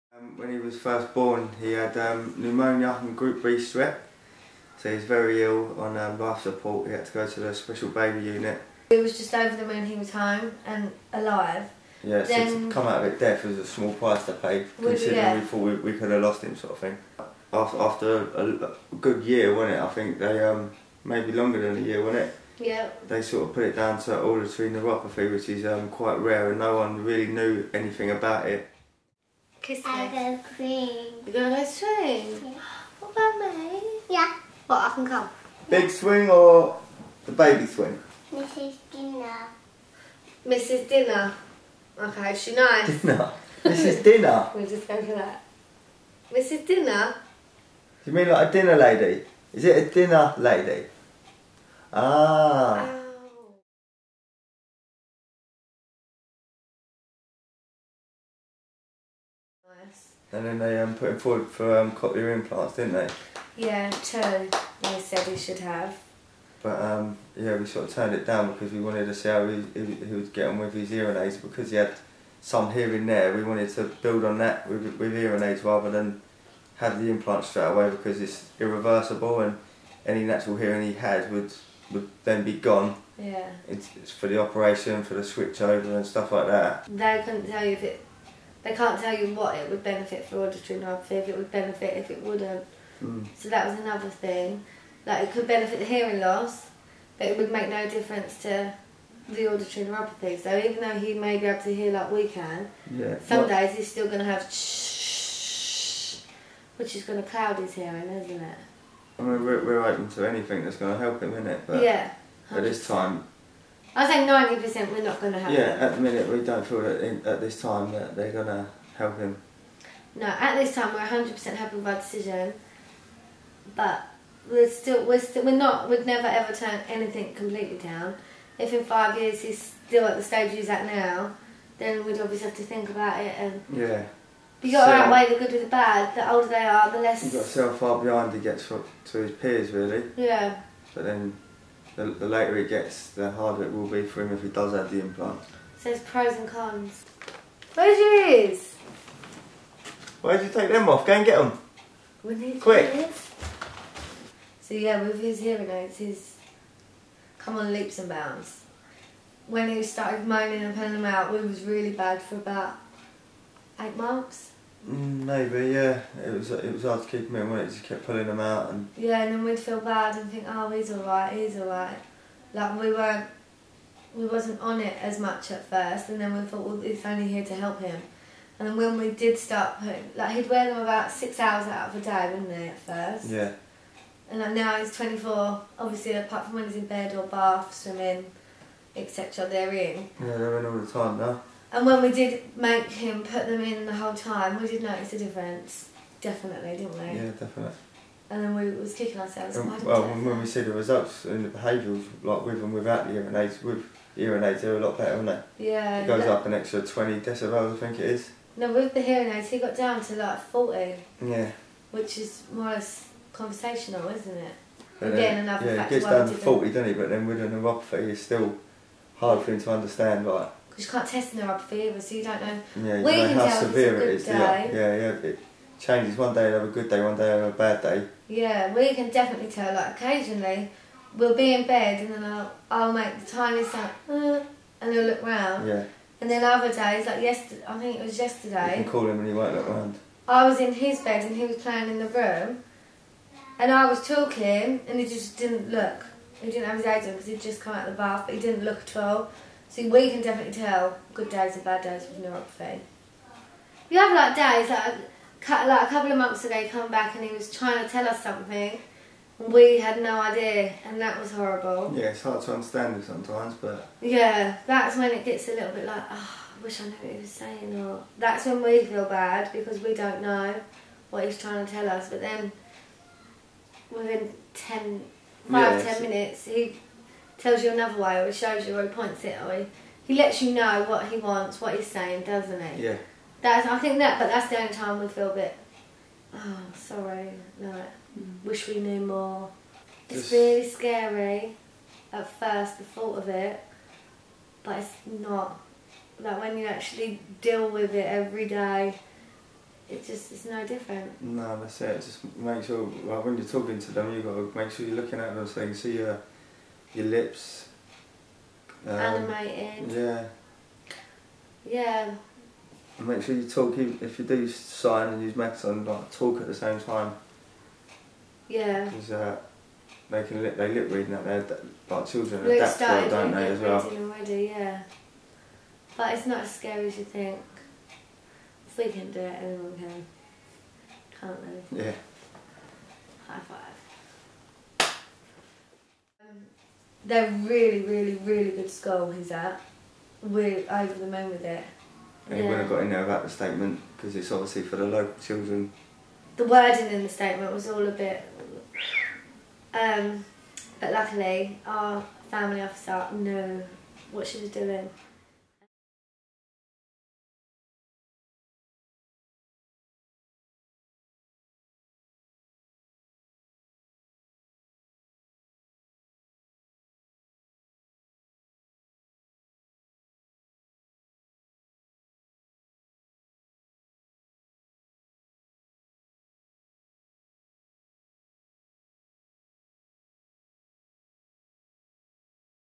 Auditory neuropathy: hearing aids and cochlear implants
Sophie and Luke tell us how they decided between hearing aids and cochlear implants for their son. You can find out more about hearing aids and cochlear implants on our